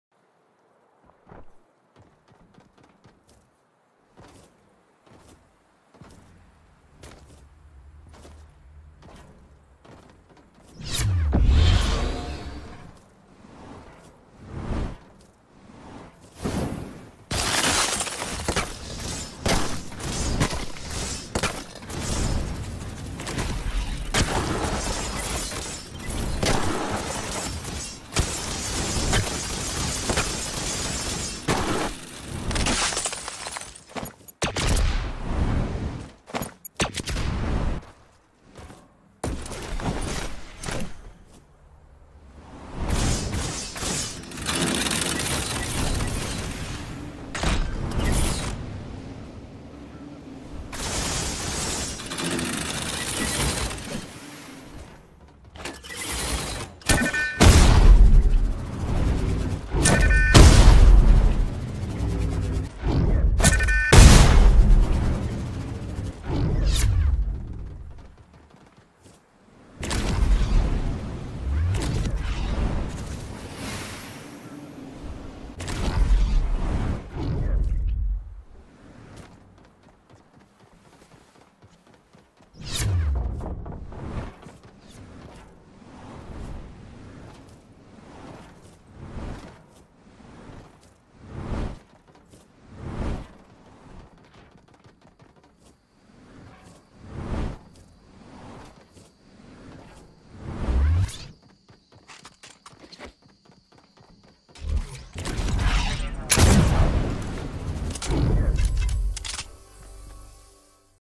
Former WORLD RECORD JDuth Default Deathrun 2 - 1:46.239
Thank you JDuth for this sick map :D

Player: Me (KoNter_UAV) |Playing since the end of season 3|

Console:                                            PS4                                                                                                                                            Server:                                               EU                                                                                                                                              Highest Solo vs Solo kill round:    22 Kills